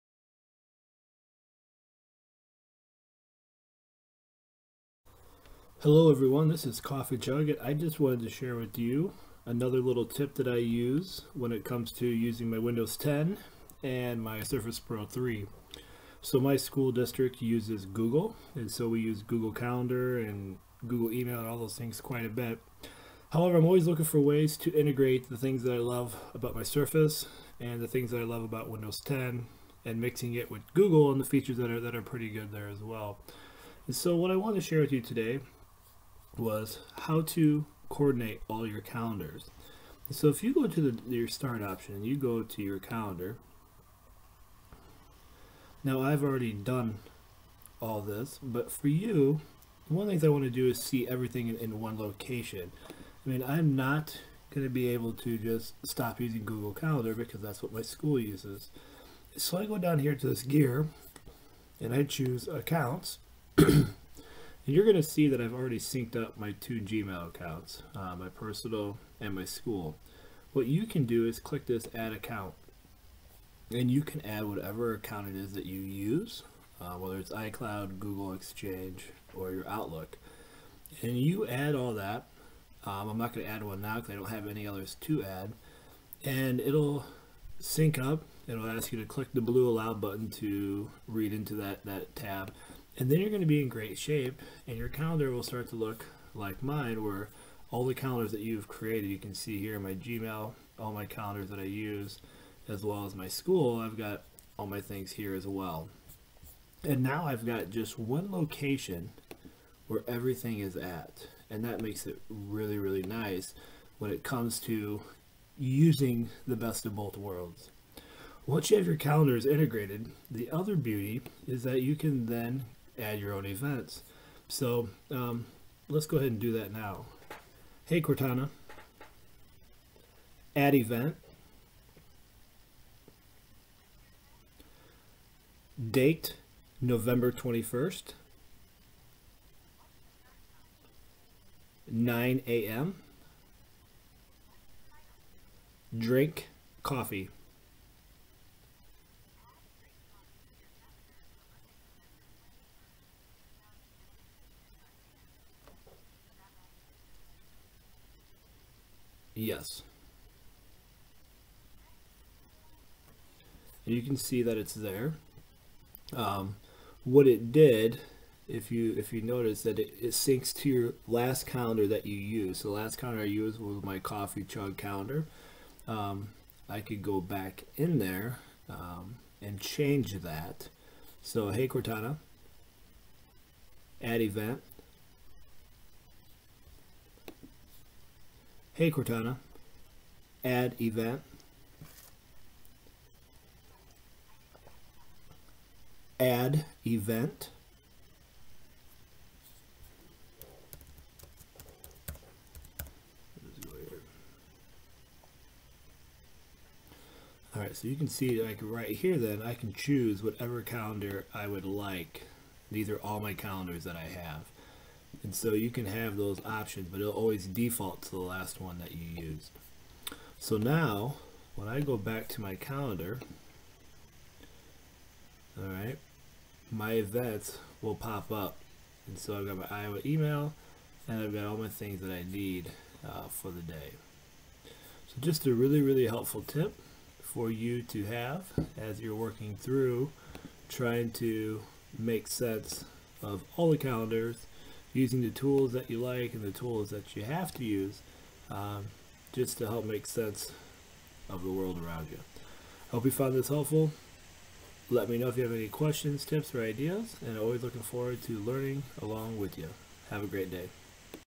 Add Calendars to Windows 10
This video covers how to add Google Calendars and others to a Windows 10 device like Surface Pro 3. You can then use Cortana to set events and use the calendar app to integrate all tools in your life.